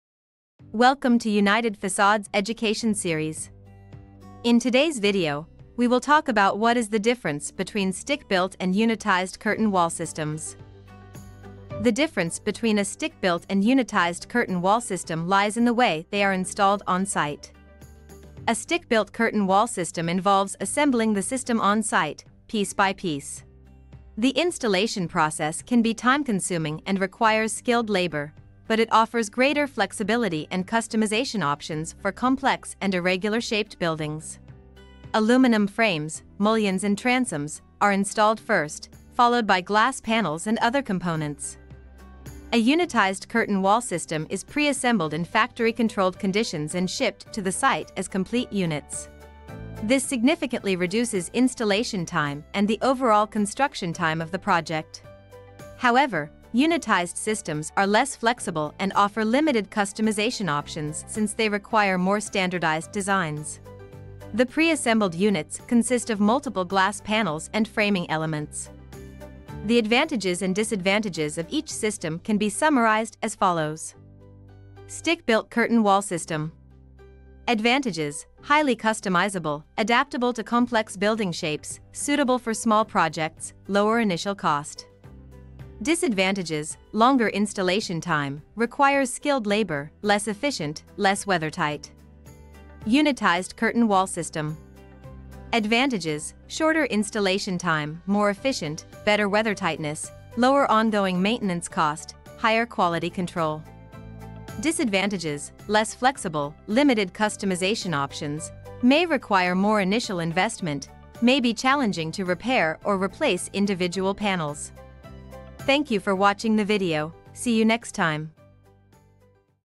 The Difference Between Stick Built and Unitized Curtain Wall Systems
Curious about curtain wall systems? In this video, we'll explore the key differences between unitized and stick-built curtain wall systems, helping you understand their installation methods and advantages.

In the world of curtain wall systems, there are two main classifications:
👉unitized curtain walls
👉stick-built systems

Unitized systems involve pre-fabricated units assembled on-site, offering quick installation and superior performance. Stick-built systems, on the other hand, are installed piece by piece, making them cost-effective for specific projects.

Join us as we delve into the details of these systems and discover their unique features.